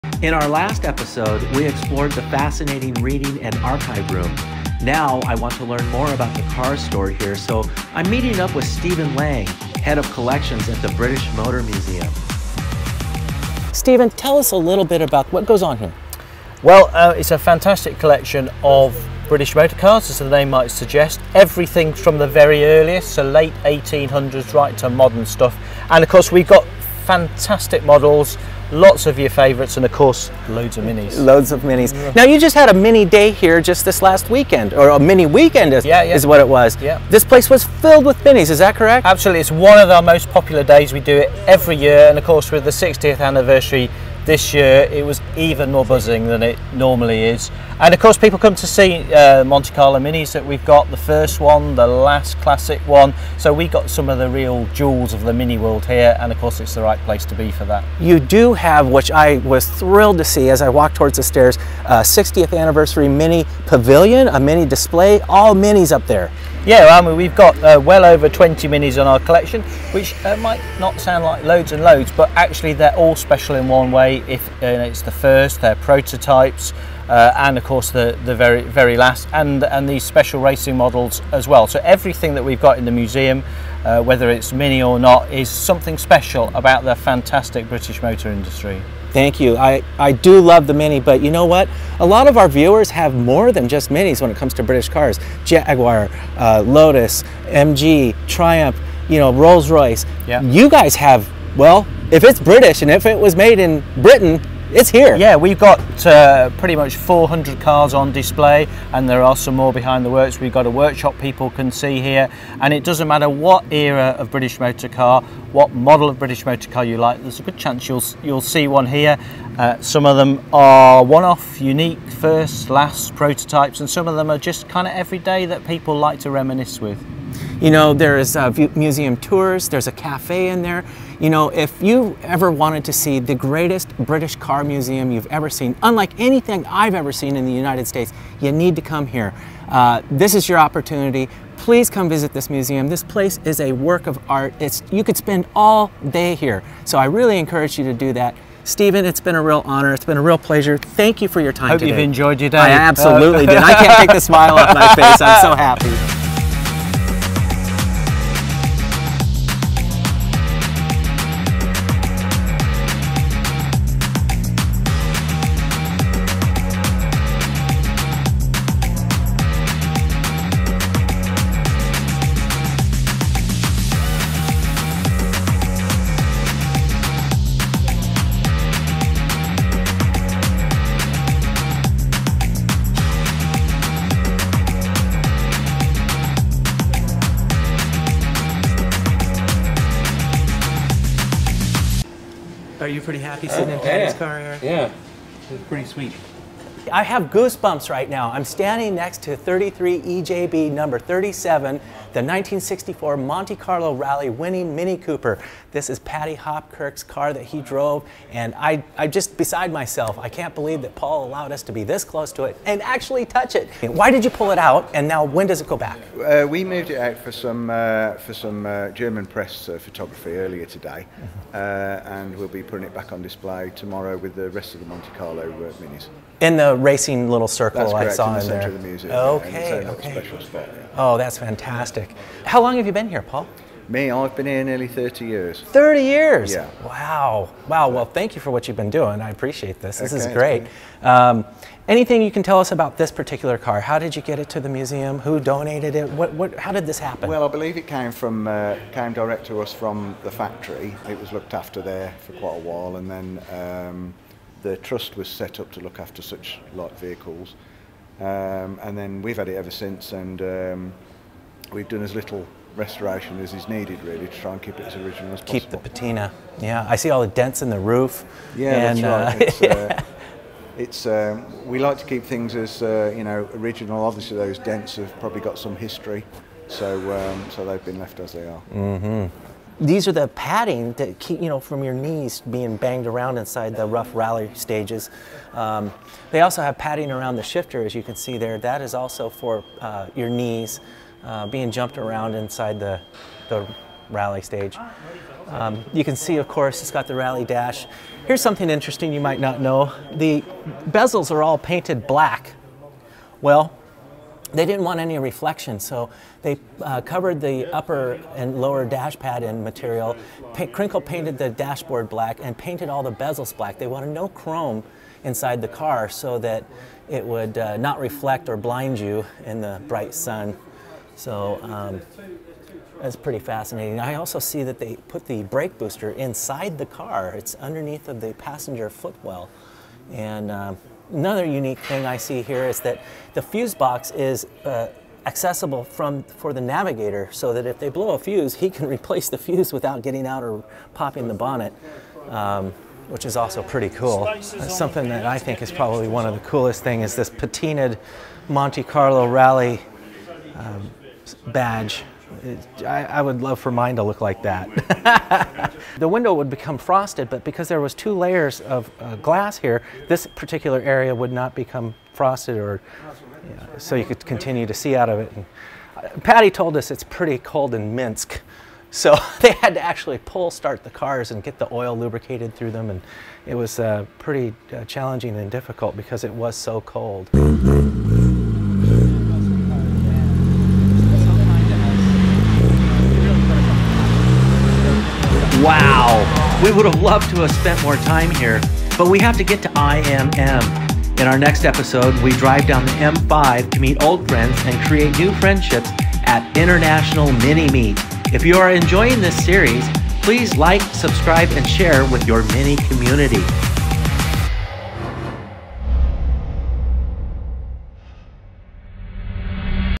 Stephen Laing Interview and up close and personal with Paddy Hopkirk's Morris Mini Cooper S 33 EJB
We continue to visit the British Motor Museum in Warwickshire, England. This museum holds the World's largest collection of historic British cars. There are over 300 classic cars on display from the British Motor Industry Heritage Trust and the Jaguar Heritage Trust.
We interview with Stephen Laing, the curator of the British Motor Heritage Museum and get "Up close and personal" with Paddy Hopkirk's Morris Mini Cooper S 33 EJB.